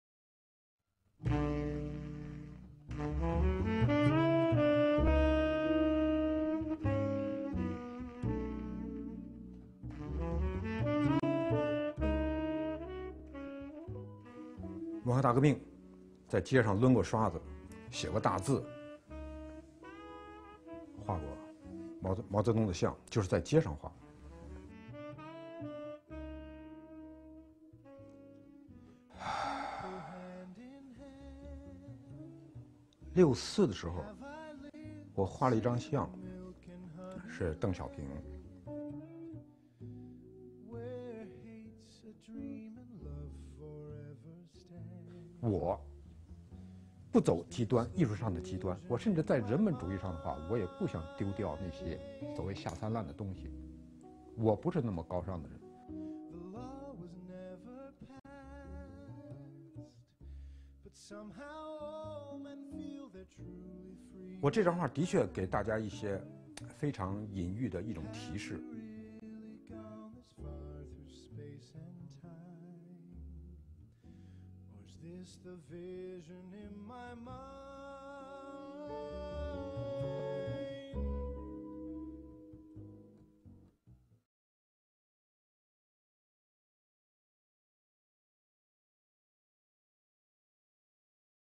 A Conversation With Artist Lui Liu
El Encanto Oculto De La Vida

Born in Tianjin in 1957, Canadian-Chinese artist Lui Liu graduated from the China Central Academy of Fine Arts, along with classmates Chen Danqing and Yang Feiyun. In 1991, he moved to Canada, where he established himself as an international artist, actively exhibiting in various museums and shows. The watercolor artist is known for his controversial works "Mud Girl", "The Legend of Warlords", and "Beijing 2008", also known as "Mahjong Women", which generated great interest and debate within the Chinese community and international circles.